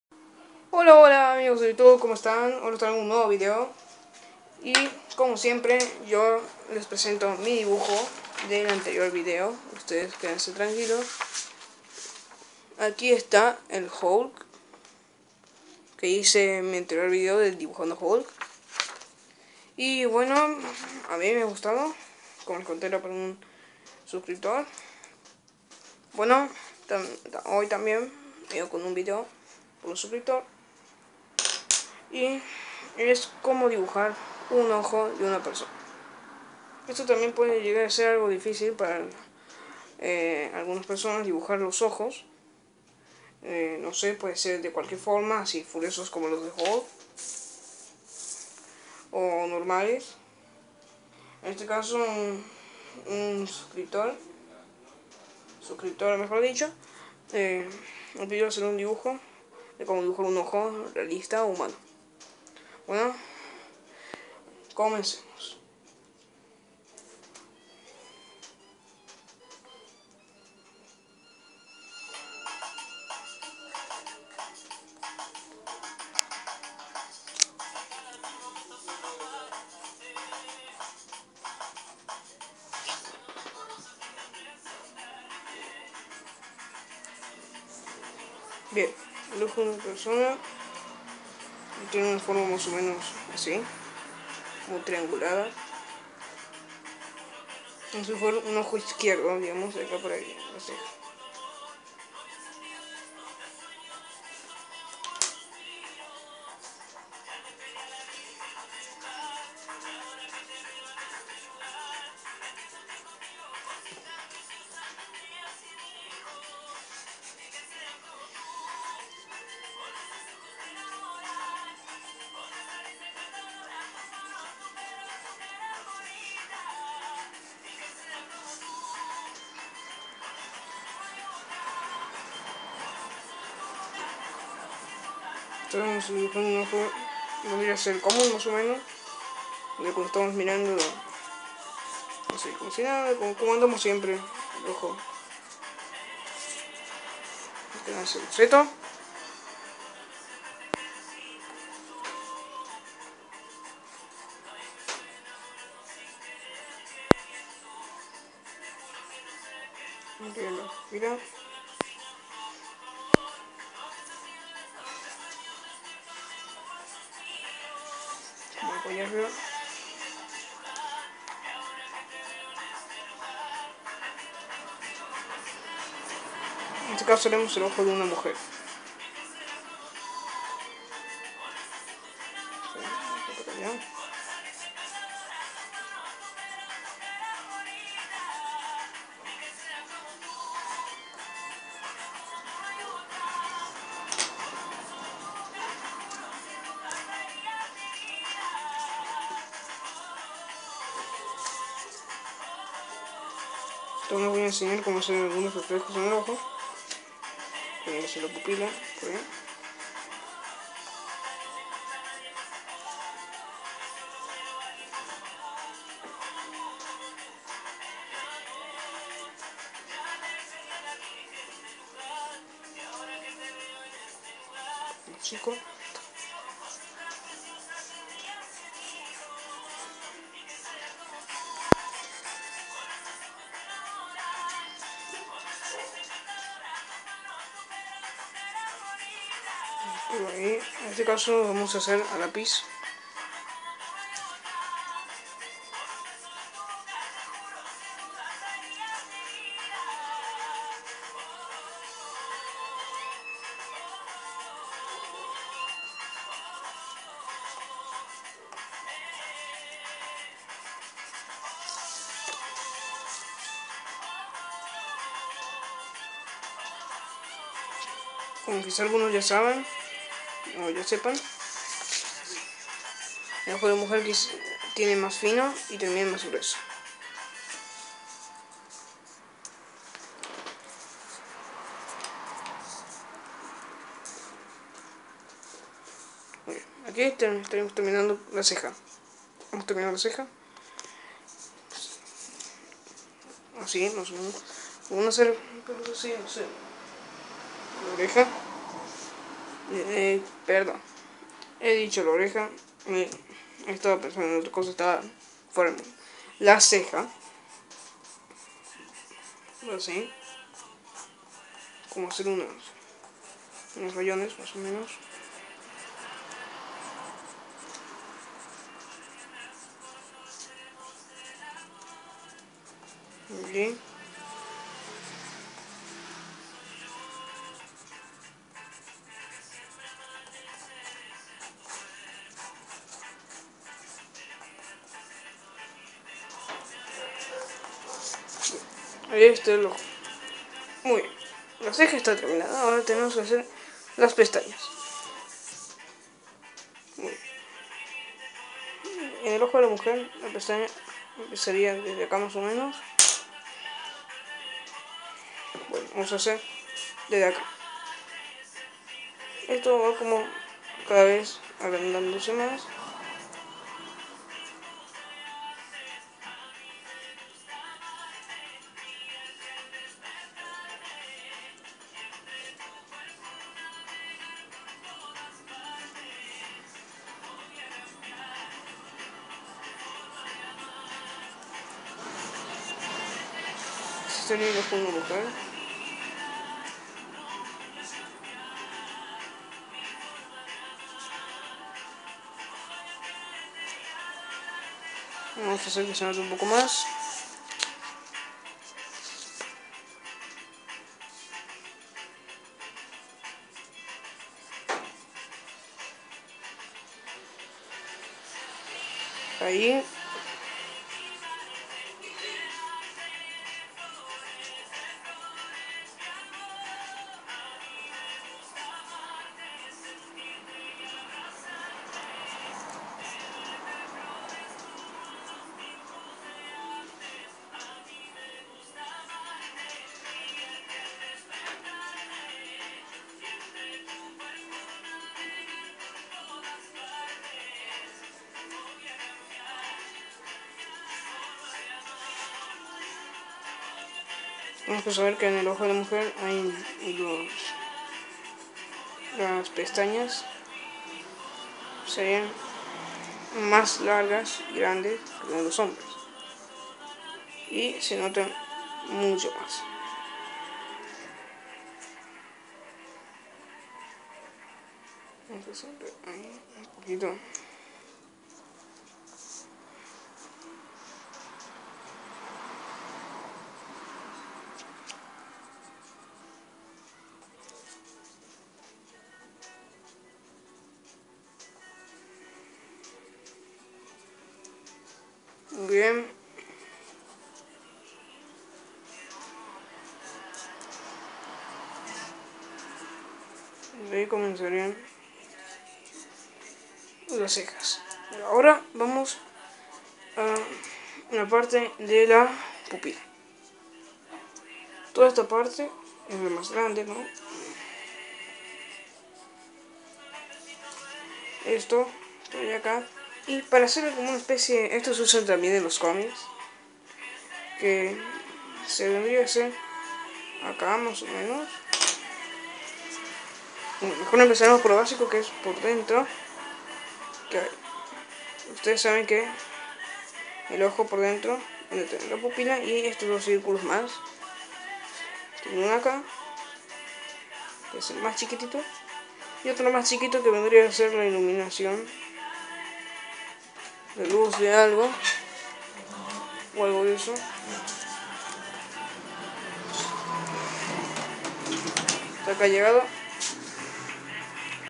como dibujar un ojo DE CABEZA¡¡
en este vídeo dibujare un ojo humano pero de cabeza por eso el vídeo esta al revés pronto are otro vídeo en competencia con este en el que dibujare otro ojo pero no de cabeza osea no al revés sino que podan verlo bien luego les explicare a casi me olvidaba pueden ver el dibujo en muy buena calidad en mi blog gracias